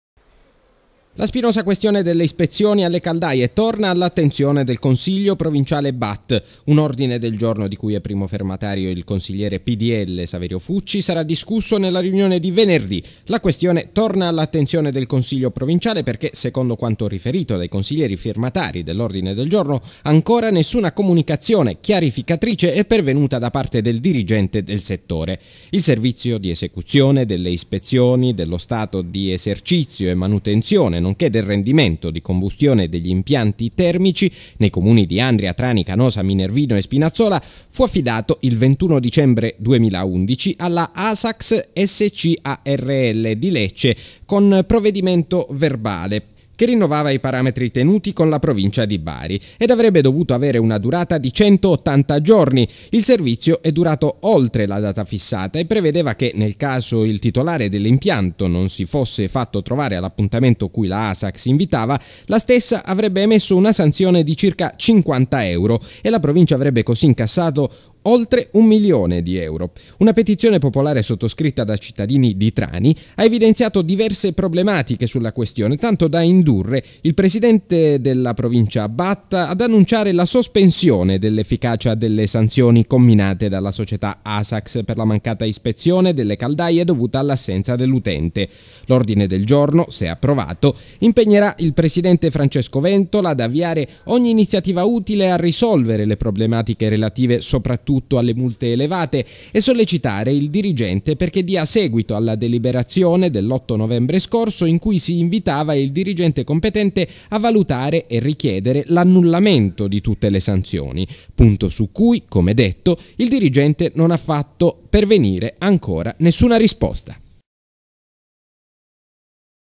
Annullamento sanzioni per impianti termici: domani odg in consiglio regionale
Ancora nessuna risposta da parte del dirigente della Provincia Bat sull'annullamento delle sanzioni derivate dalla mancata presenza dell'utente alla verifica degli impianti termici: per questo alcuni consiglieri provinciali hanno proposto un ordine del giorno che sarà discusso in consiglio nella riunione di domani.